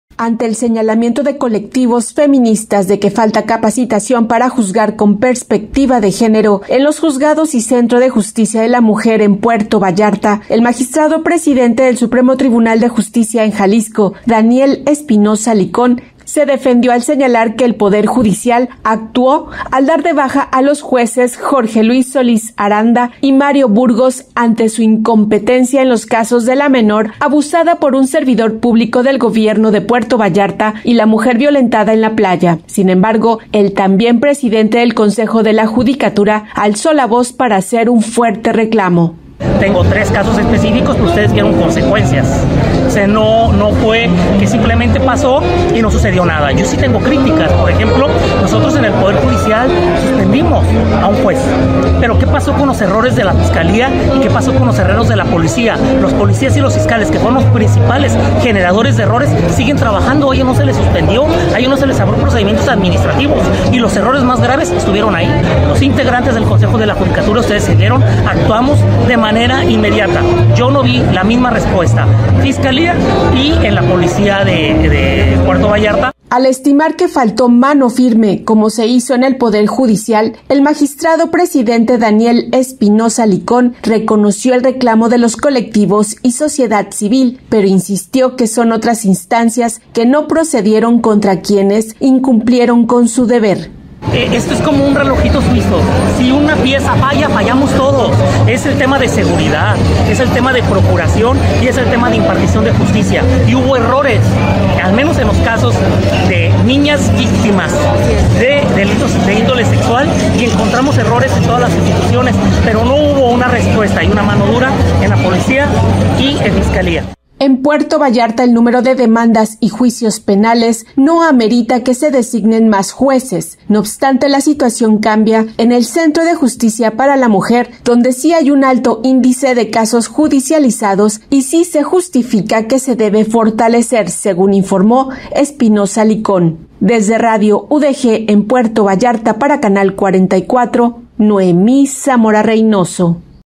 Daniel Espinosa Licon, denunció que la FGE y Policía Municipal no procedieron con esa dureza.
El presidente magistrado del supremo tribunal de justicia en Jalisco y consejo de la judicatura en Jalisco, Daniel Espinosa Licon, denunció que, tras la suspensión de tres jueces en Puerto Vallarta, la Fiscalía de Jalisco y Policía Municipal no procedieron con esa dureza ante casos de violencia contra niñas, adolescentes y mujeres que siguen exigiendo justicia.